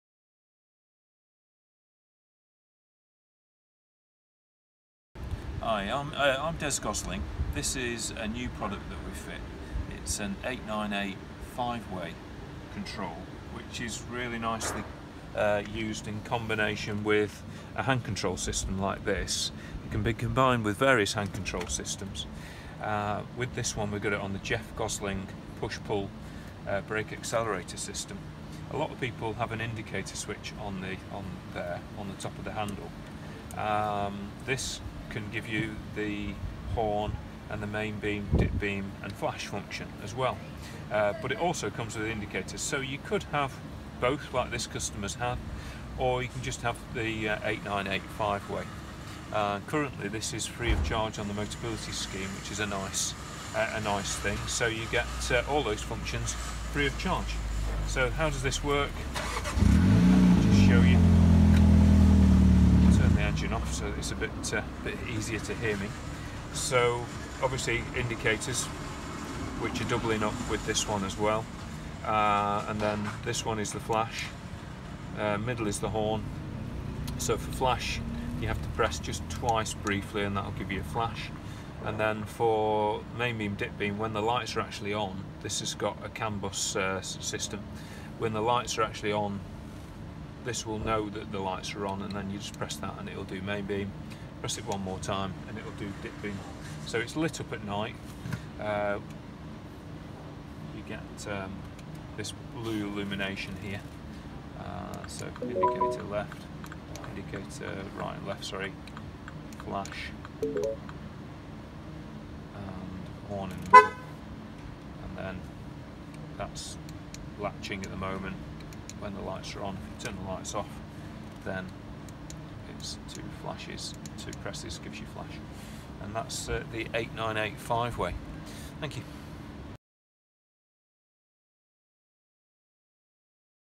5 Way Remote Control Device
The 5 Way Remote Control Device is a small round keypad, positioned on the right hand side of the steering wheel, which allows you to activate headlights, indicators and horn without having to release your hold on the steering wheel.